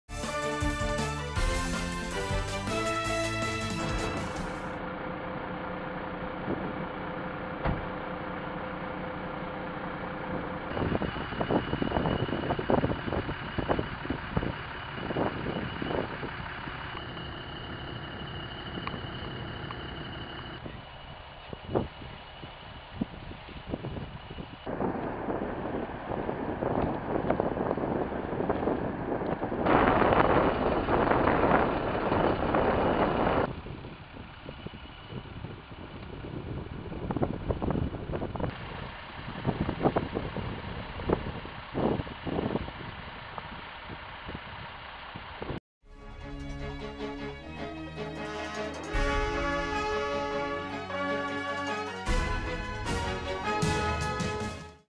Śmierć na Łączniku
W tragicznym wypadku zginął wczoraj 28-letni mieszkaniec Wałbrzycha.